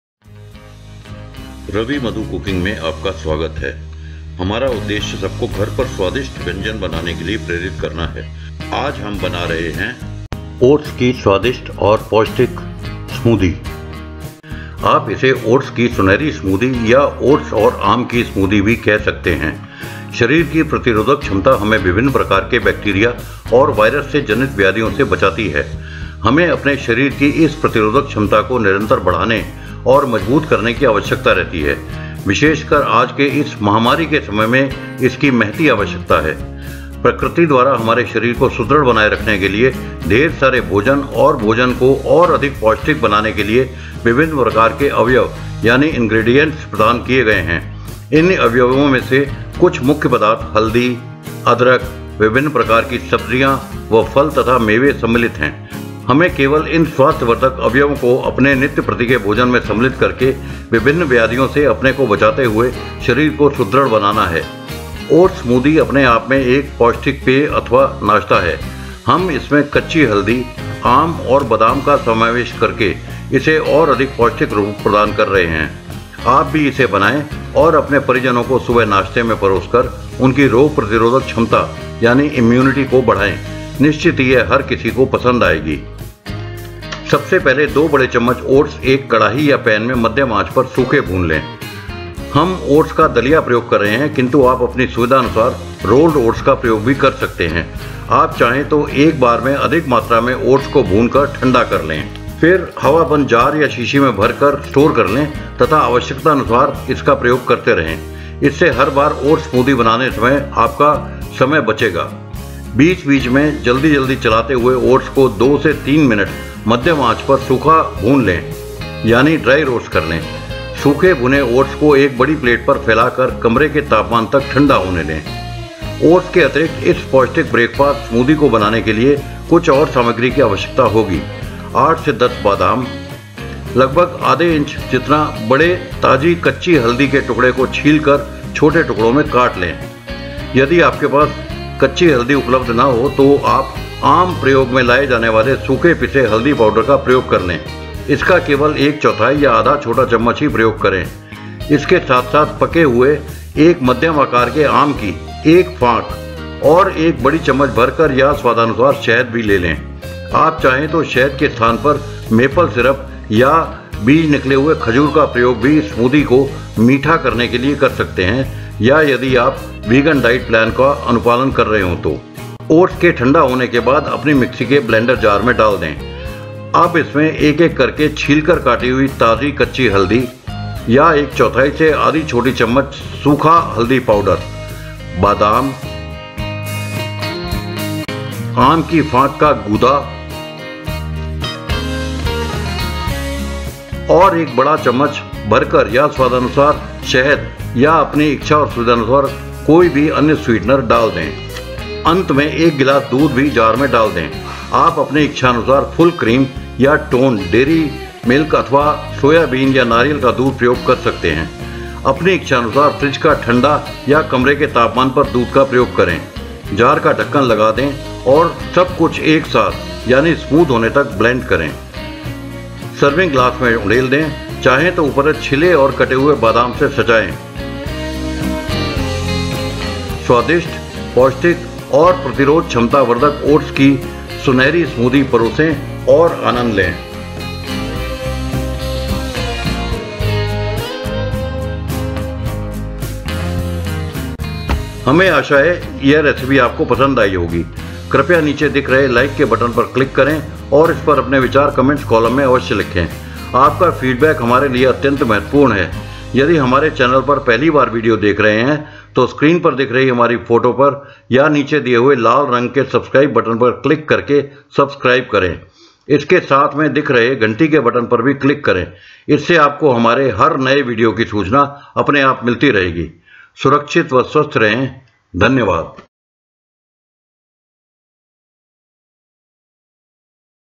ओट्स की स्वादिष्ट और पौष्टिक स्मूदी - Golden oats smoothie – Boost Your Immunity in Delicious Way
आप इसे ओट्स की सुनहरी स्मूदी या ओट्स और आम की स्मूदी भी कह सकते हैं। शरीर की प्रतिरोधक क्षमता हमें विभिन्न प्रकार के बैक्टीरिया और वायरस से जनित व्याधियों से बचाती है। हमें अपने शरीर की इस रोग प्रतिरोधक क्षमता को निरंतर बढ़ाने और मजबूत करने की आवश्यकता रहती है, विशेषकर आज के इस महामारी के समय में इसकी महती आवश्यकता है। प्रकृति द्वारा हमारे स्वास्थ्य को सुदृढ़ बनाए रखने के लिए ढेर सारे भोजन और भोजन को और अधिक पौष्टिक बनाने के लिए विभिन्न प्रकार के अवयव यानि ingredients प्रदान किए गए हैं। इन अवयवों में से कुछ मुख्य पदार्थ हल्दी, अदरक, विभिन्न प्रकार के फल तथा मेवे आदि सम्मिलित हैं। हमें केवल इन स्वास्थ्यवर्धक अवयवों को अपने नित्यप्रति के भोजन में सम्मिलित करके विभिन्न व्याधियों से अपने को बचाते हुए शरीर को सुदृढ़ बनाना है।
ओट्स स्मूदी अपनेआप में एक पौष्टिक पेय अथवा नाश्ता है। हम इसमें कच्ची हल्दी, आम और बादाम का समावेश करके इसे और अधिक पौष्टिक रूप प्रदान कर रहे हैं। आप भी इसे बनाएं और अपने परिजनों को सुबह के नाश्ते में परोस कर उनकी रोग प्रतिरोधक क्षमता को स्वादिष्ट तरीके से बढ़ाएं। निश्चित ही यह हर किसी को पसंद आएगी।

Recipe:

Serves: 1

Ingredients:

• 2 tbsp. oats (porridge or rolled) (२ बड़े चम्मच ओट्स का दलिया या रोल्ड ओट्स),
• ½ inch large piece of fresh turmeric, peeled and chopped (छीलकर काटे हुए ताज़ी हल्दी के टुकड़े),
• 8 to 10 almonds (८ से १० बादाम),
• 1 or 2 slices of fresh ripe mango (मध्यम आकार के पके हुए आम की एक फांक),
• 1 tbsp., or to taste honey, any other sweetener of choice (१ बड़ी चम्मच या स्वादानुसार शहद),
• 1 glass milk (Any – full cream or skim or soy/coconut milk) (डेयरी/सोयाबीन या नारियल का १ गिलास दूध),

Method:

1. Dry roast 2 tbsp. oats in wok over medium heat, we are using oats porridge but you may use rolled oats as well. You may also roast larger quantity of oats at a time and store in airtight jar after cooling down to room temperature, it will save your time every time you need to make oats smoothie. Dry roast oats while stirring frequently for 2 to 3 minutes. Spread dry roasted oats over a plate until cooled down to room temperature.
2. Add cooled roasted oats into your blender jar along with peeled and chopped fresh turmeric or ¼ tsp dried turmeric powder, and almonds; also scoop pulp from ripe mango slice and add into blender. Add 1 tbsp. or to taste honey or sweetener of choice into jar.
3. Pour 1 glass of milk, use chilled milk or at room temperature according to your liking; close the jar with its lid and blend well until everything is well blended and smooth.
4. Pour into serving glass, garnish with peeled or slivered sliced almonds if like to. Serve delicious healthy and immunity boosting golden oats smoothie and enjoy.